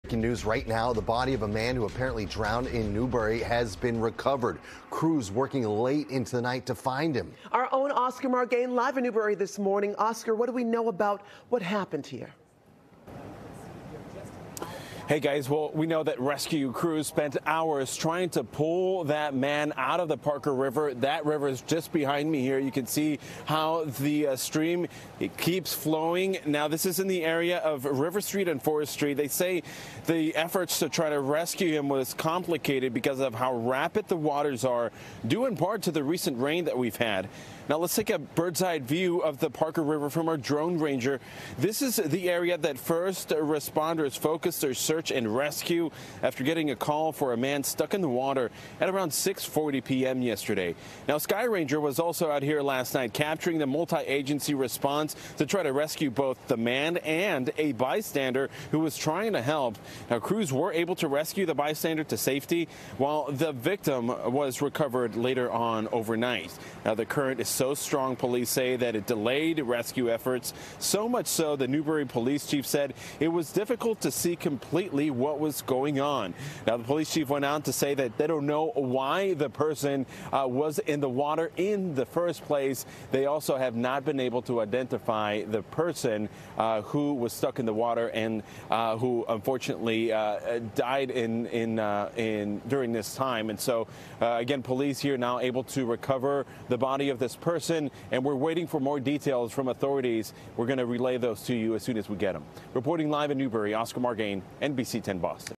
Body of man recovered from river in Newbury
A man's body was recovered from a fast-moving river in Newbury, Massachusetts, on Friday, police say.